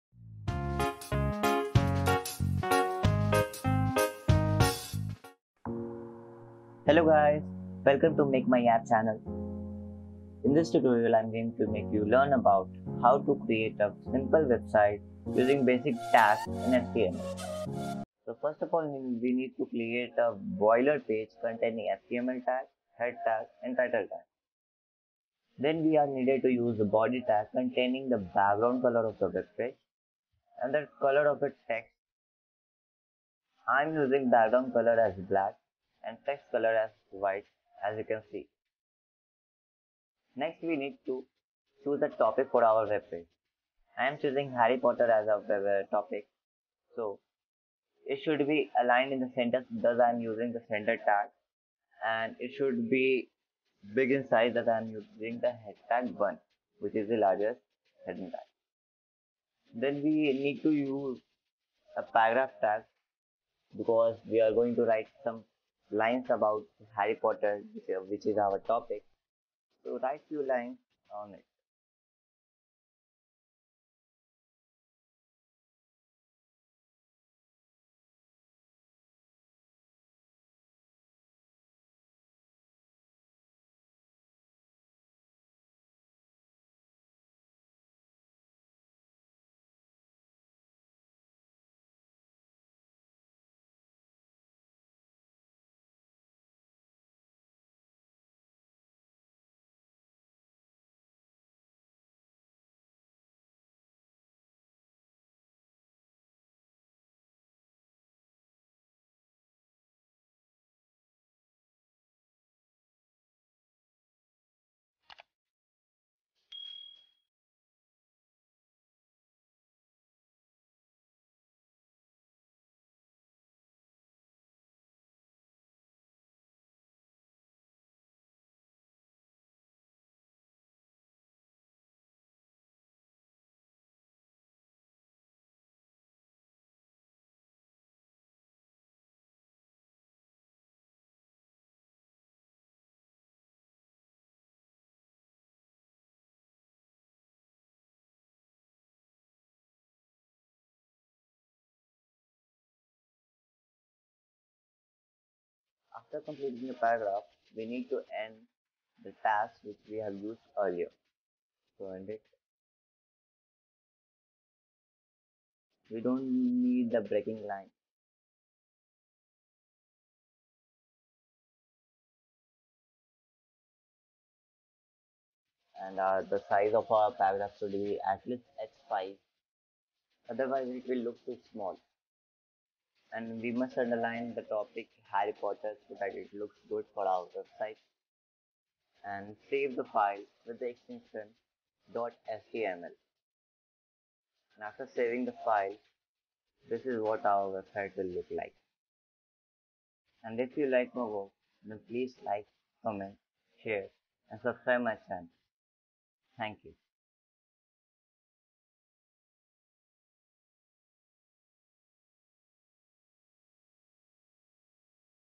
HTML Tutorial for beginners | Simple Website HTML | Make My App
This is my first voice over a video which is about creating a simple website using some basic tags in HTML.